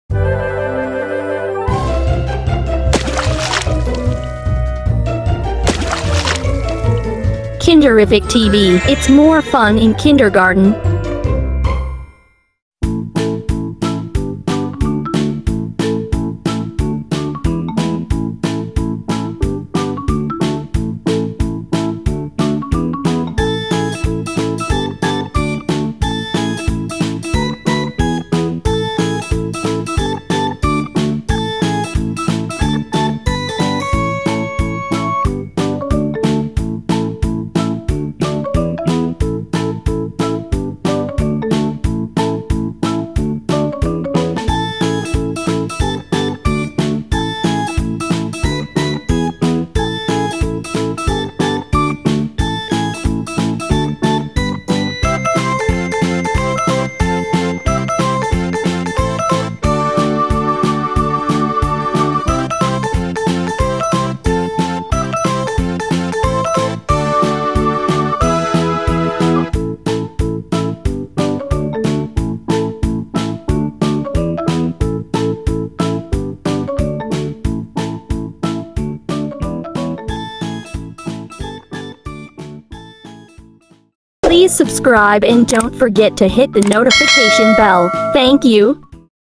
MELC BASED QUARTER 2 WEEK 5 ANG ATING KOMUNIDAD | ACTIVITY SHEET | KINDERIFIC TV
Kindergarten Most Essential Learning Competencies

•Name the places and the things found in the classroom, school and community.

• Tell that the quantity of a set of objects does not change even though the arrangement has changed (i.e., the child should be able to tell that one set of counters placed in one-to-one correspondence and then rearranged still has the same quantity)

Happy Teaching😊